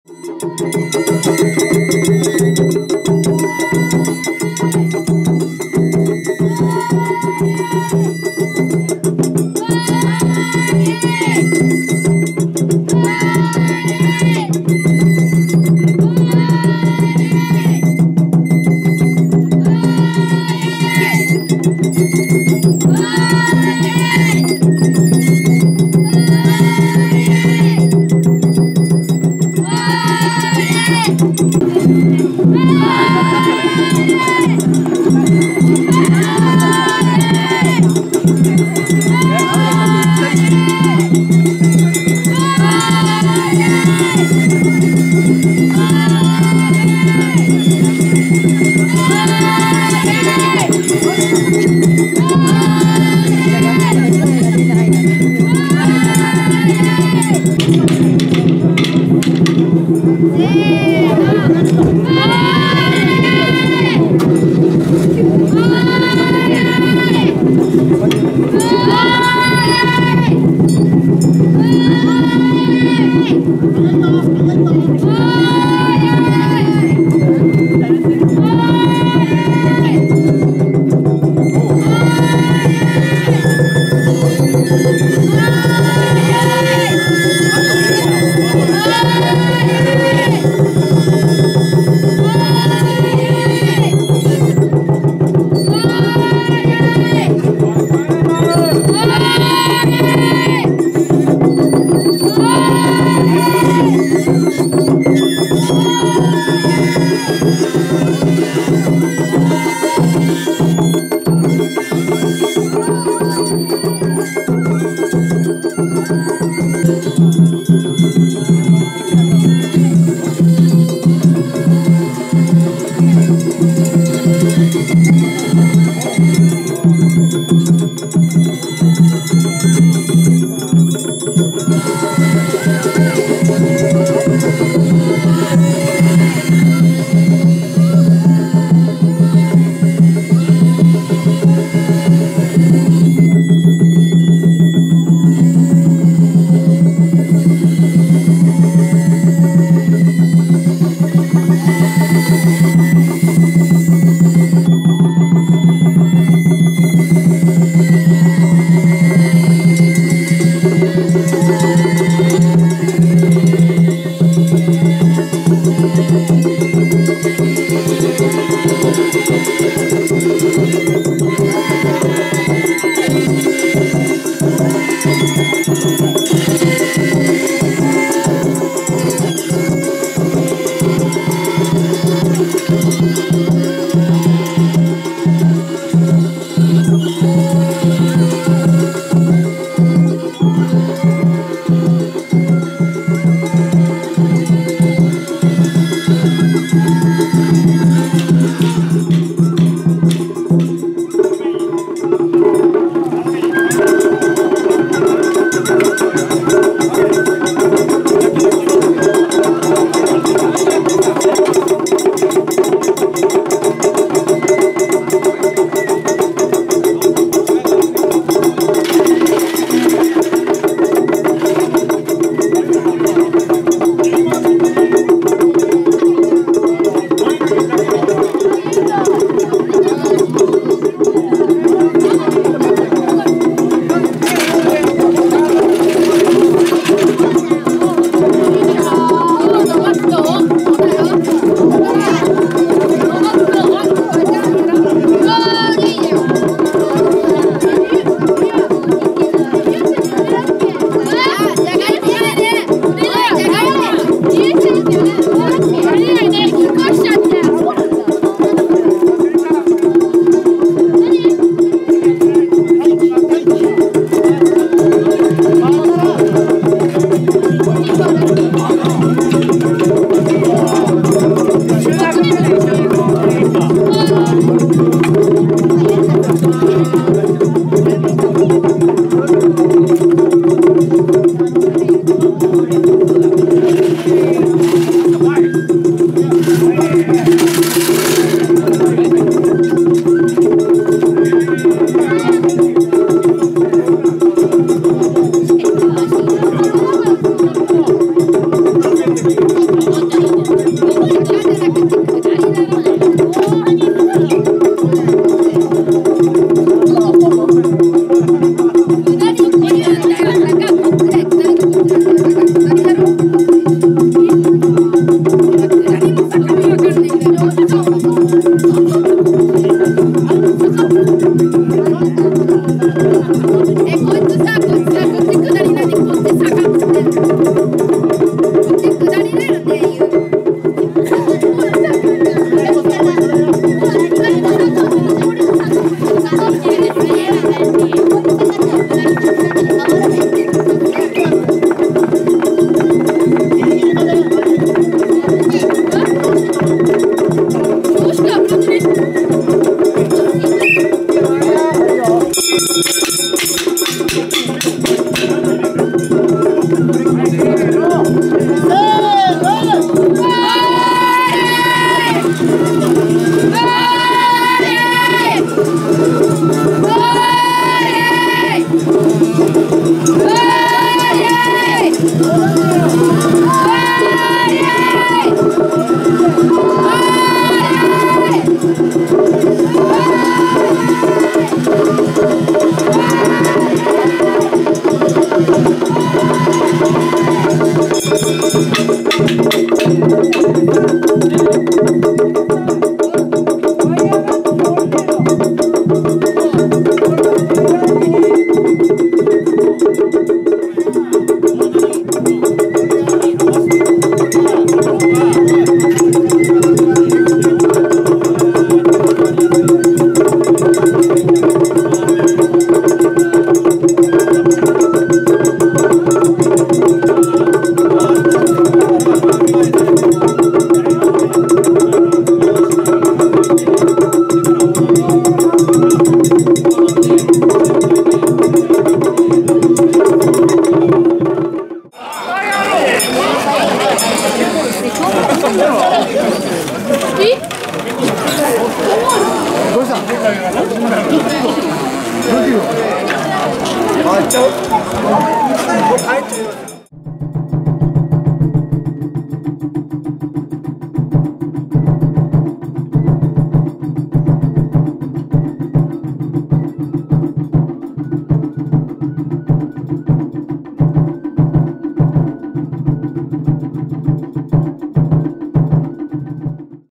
【宇根の春祭り】平成３１年 宇根の春祭り③ 〜下組笠鉾編〜
宇根の春祭りより、下組笠鉾編をお送りいたします。
　宇根の下組笠鉾は、頂きに銀の満月を掲げる特徴的な笠鉾です。

　宇根の春祭りは毎年４月の第1日曜日に開催されます。

◆平成３１年　宇根の春祭り